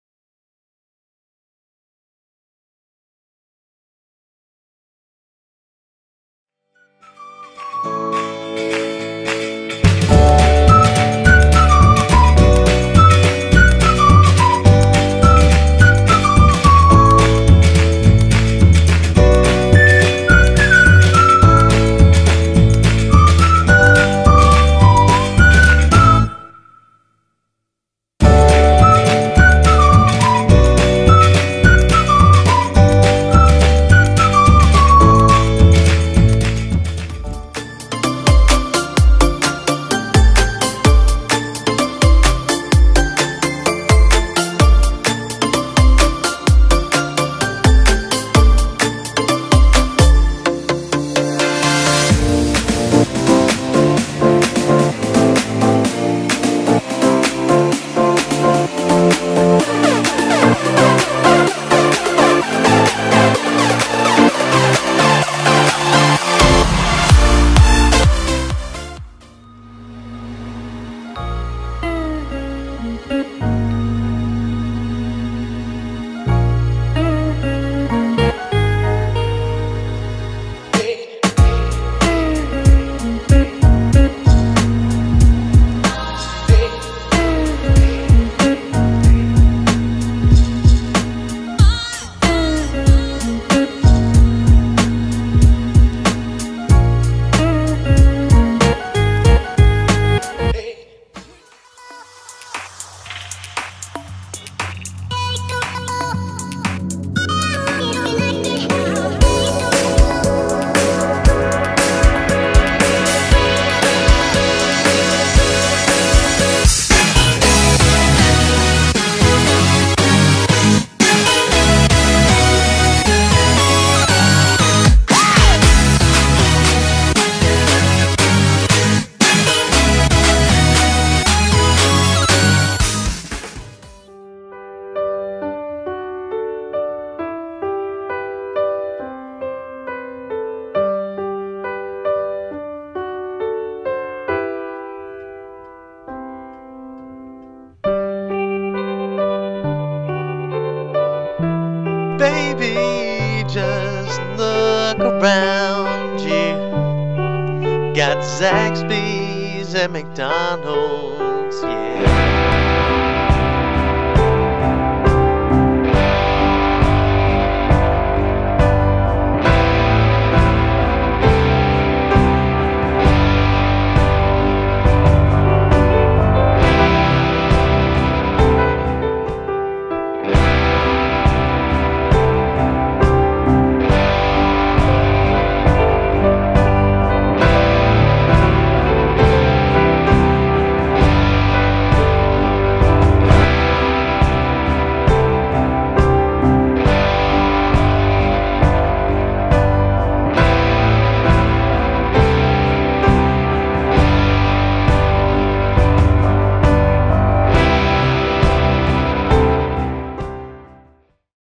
Let's Eat... Nashville, NC - Special Song at 2:25 - Kelly Kinnin
Flying around Nashville, NC with a special song at 2:25 by Kelly Kinnin